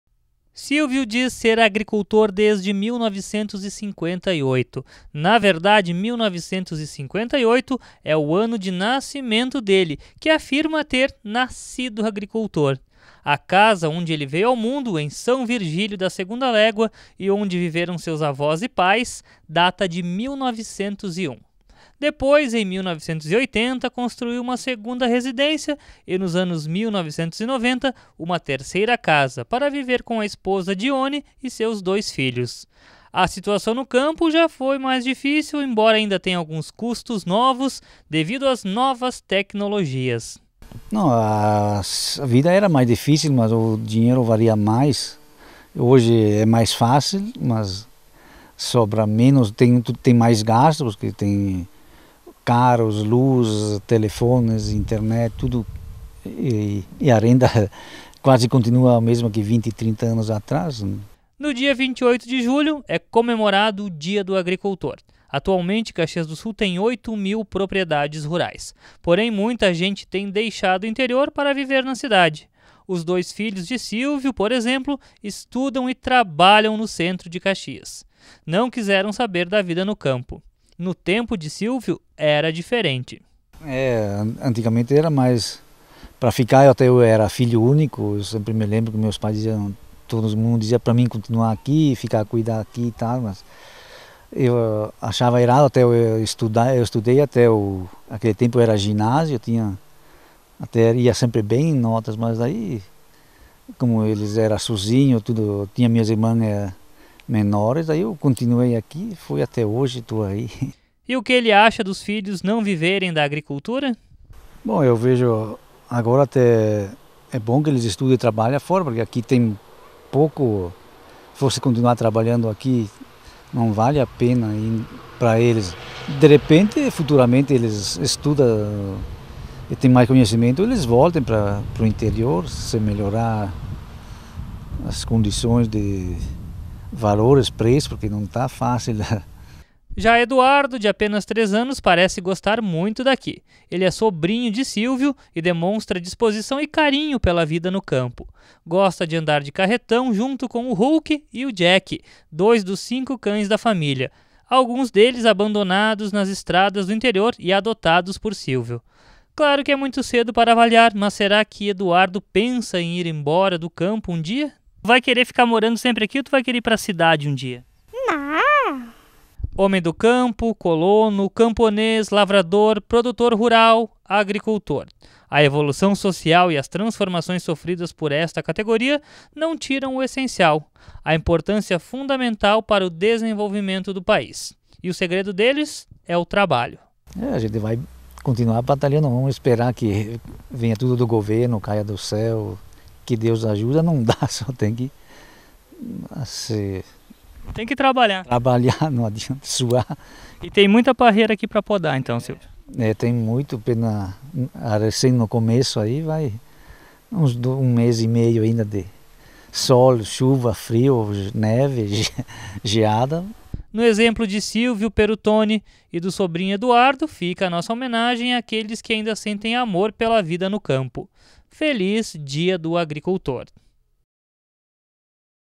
No dia do agricultor visitamos uma propriedade rural em São Virgílio da 2ª Légua
Chamado de homem do campo, colono, camponês, lavrador, produtor rural, não importa... O agricultor é fundamental no campo ou nas cidades: é ele que produz o alimento que está na mesa de cada um de nós. E este dia 28 de julho é dedicado a ele. Por isso fomos até São Virgílio da Segunda Légua visitar uma propriedade rural para mostrar um pouco do dia a dia e saber o que pensa atualmente o nosso agricultor.